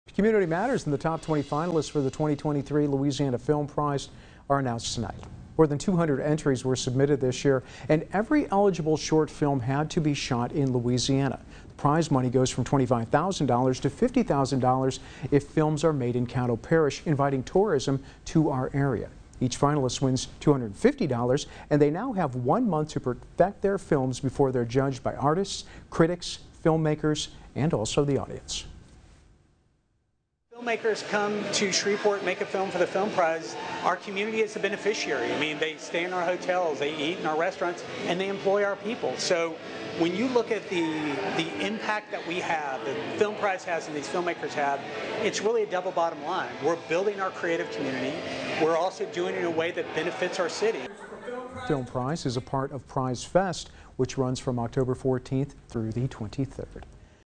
Louisiana Film Prize names top 20 finalists in Shreveport
Louisiana Film Prize is part of Prize Fest.